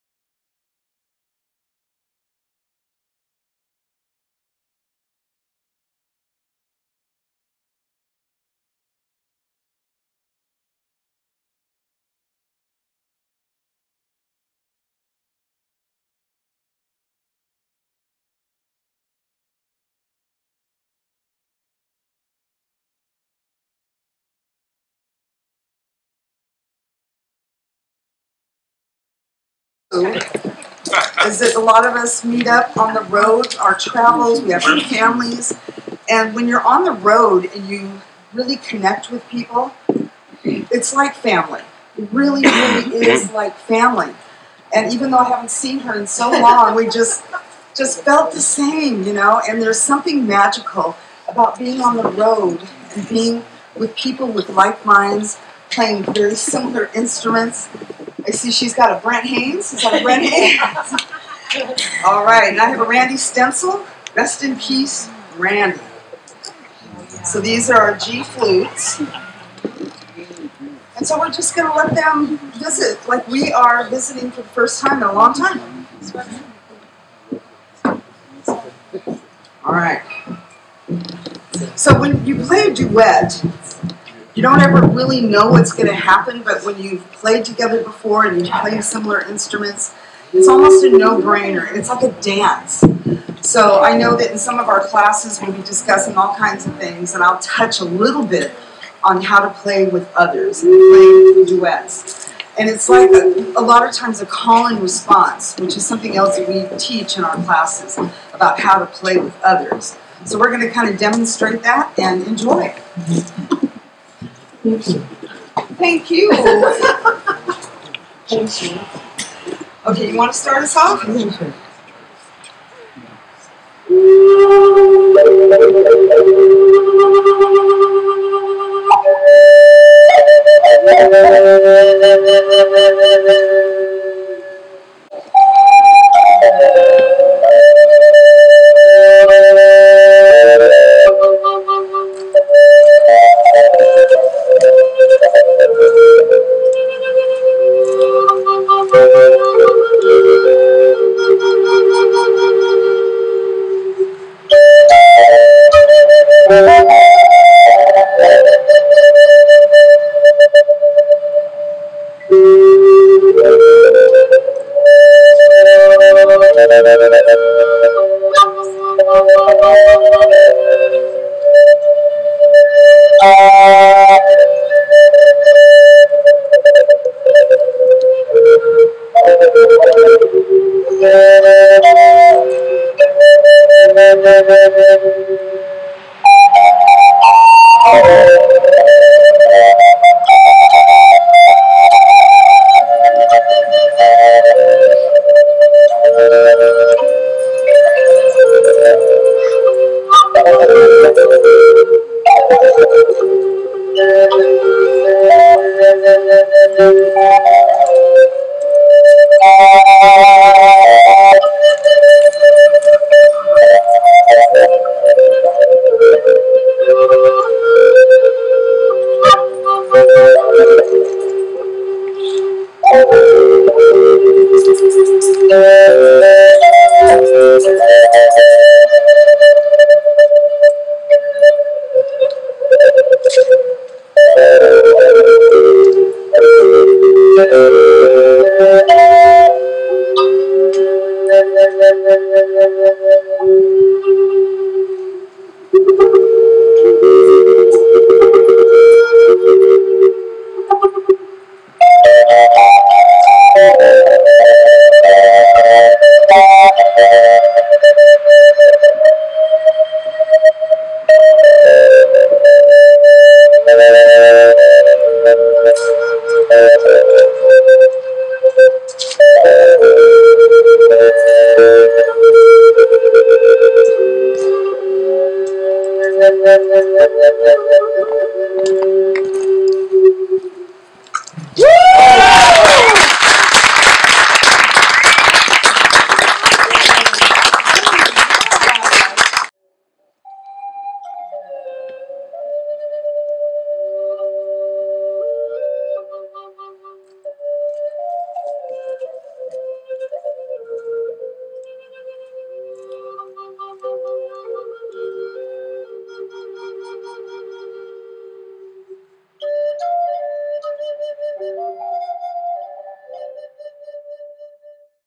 Mary Youngblood & Rona Yellow Robe play an Improvisational Duet Flutestock 2022
What a Blessing!
 Mary Youngblood playing an Improvisation with Rona Yellow Robe for us at
Flutestock Flute Festival July 15, 2022

Please visit their websites & consider purchasing some of their beautiful music to add to your collection. or add them to your playlists in your preferred streaming platform!